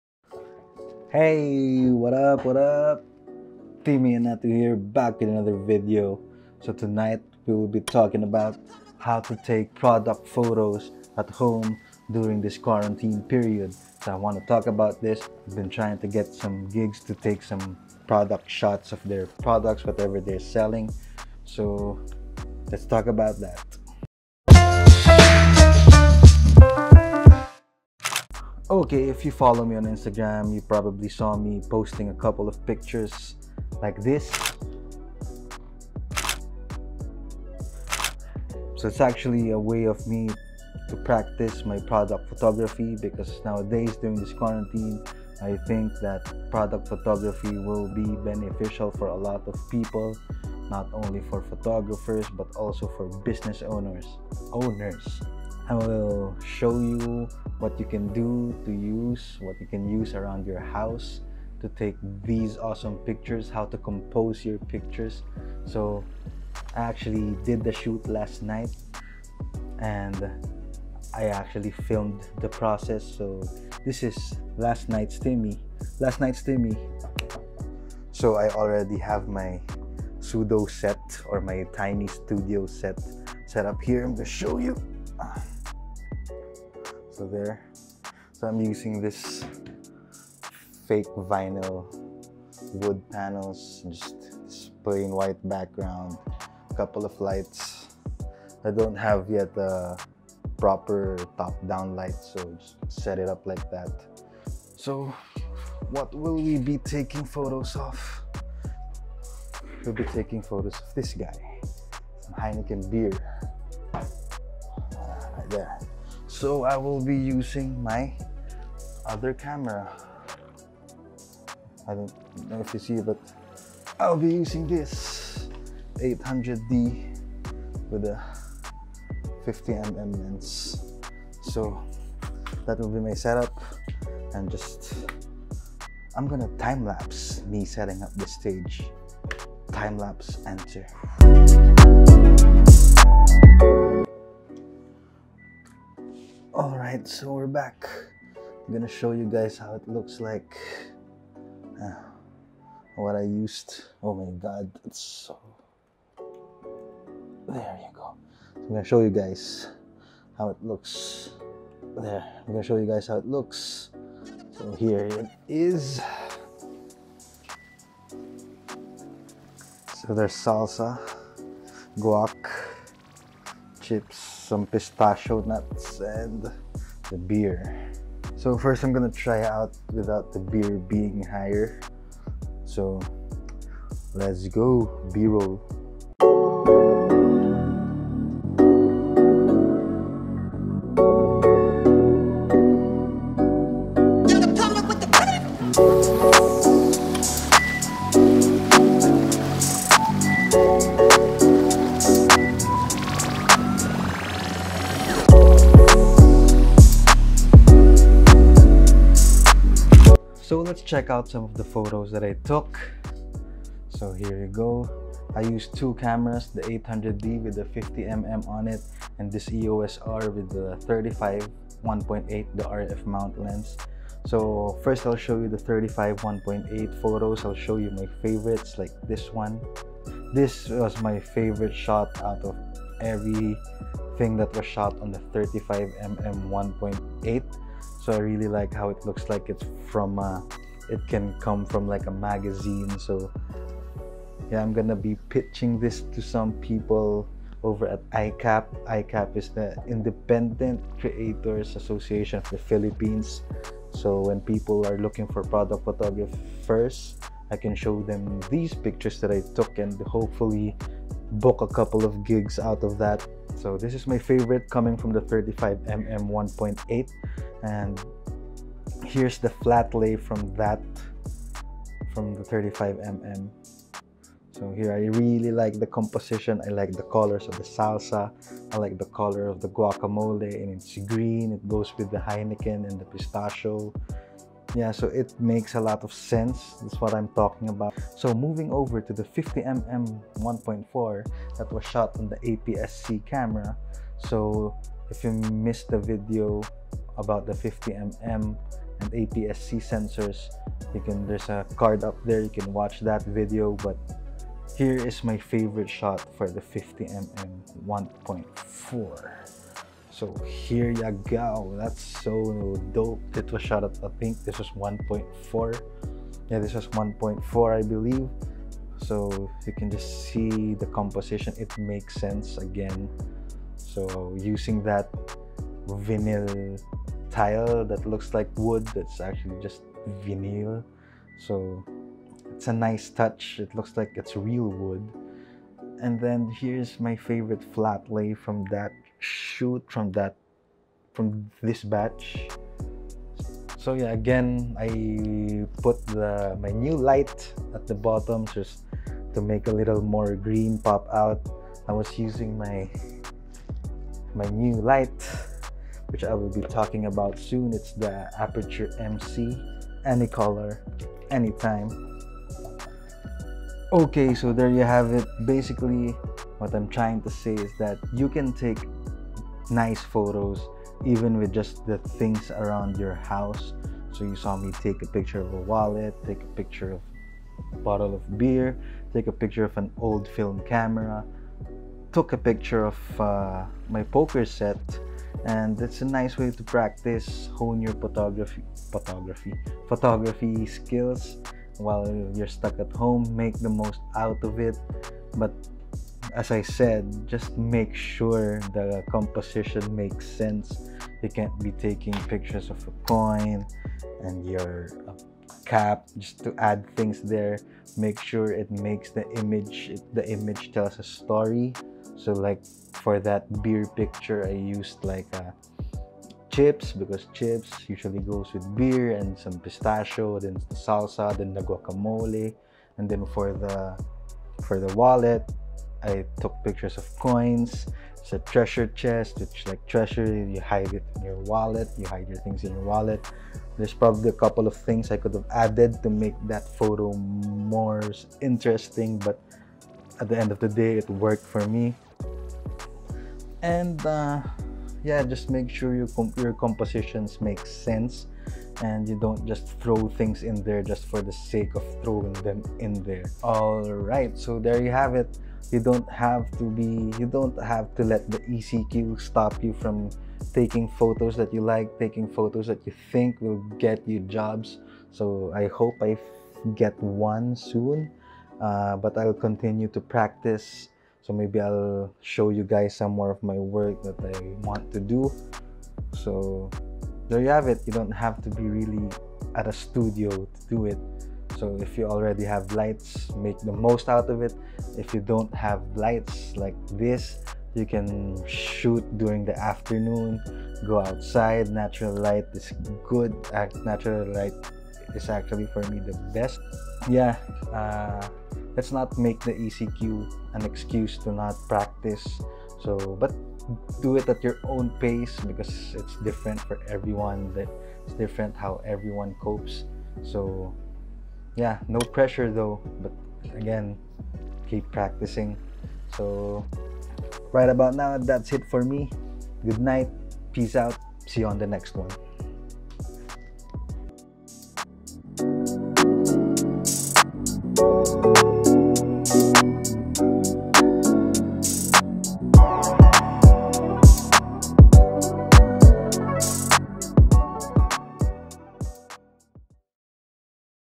HOW TO DO PRODUCT PHOTOGRAPHY AT HOME
On this video, I will show you how to step up your product photography from home with things lying around your house

Gear Used

Main Video Camera: Canon EOS R with EF 16-35MM 2.8
Photography gear: Canon 800D with 50mm 1.4 and Canon EOS R with RF 35mm 1.8